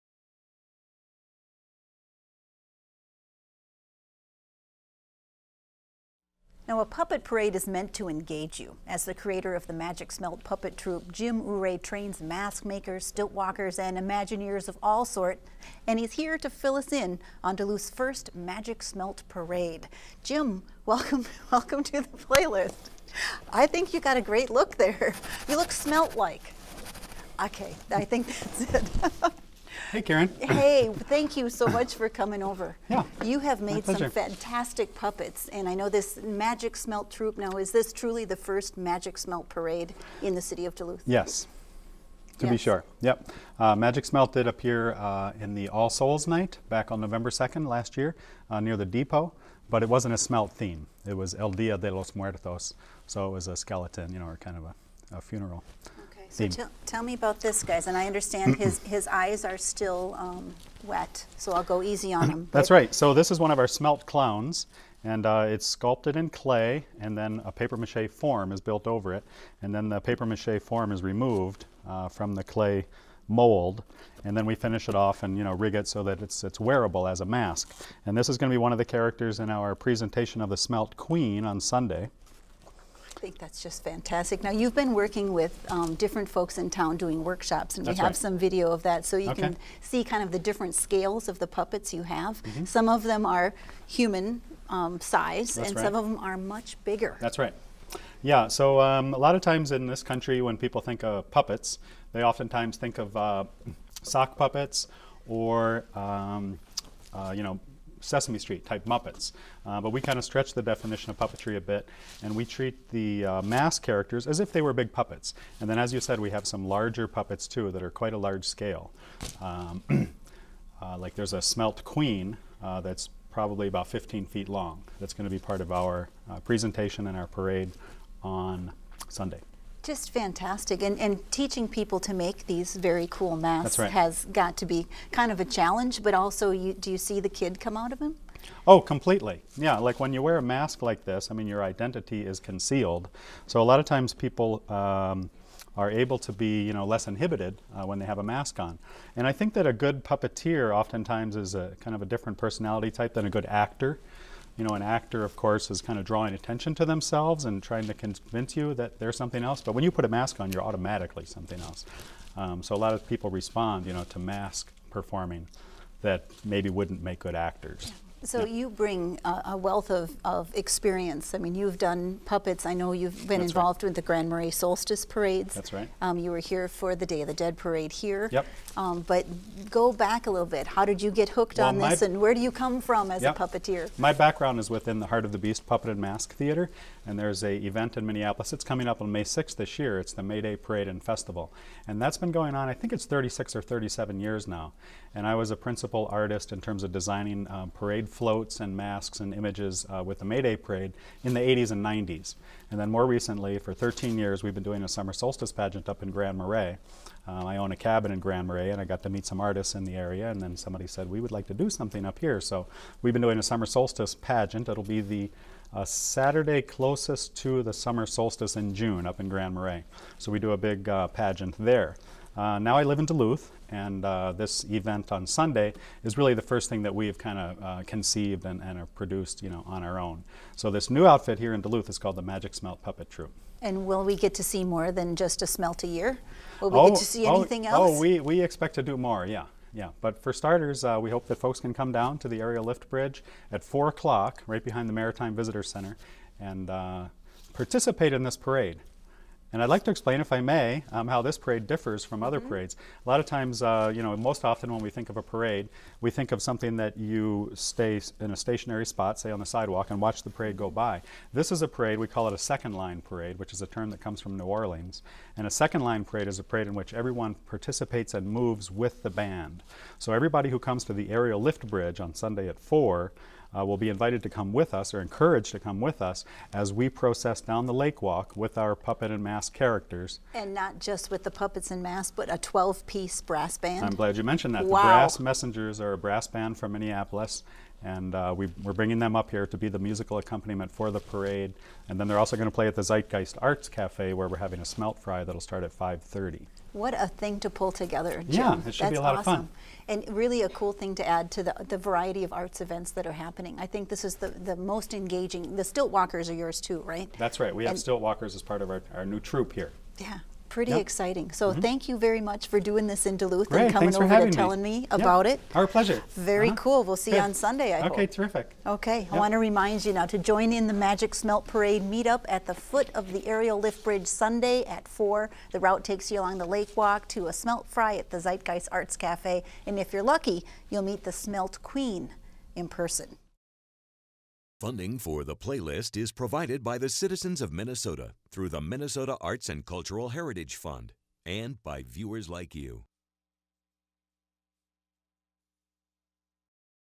Magic Smelt Puppeteer Jim Ouray
The creative genius behind the Magic Smelt Puppet Troupe shares his passion for play and puppetry.  Jim Ouray, aka Mr. Smelt, visits The PlayList in advance of the first every Magic Smelt Parade in Duluth, MN.  4 pm Sunday, April 22 at the foot of the Aerial Lift Bridge, parade to Zeitgeist Arts Cafe for smelt fry.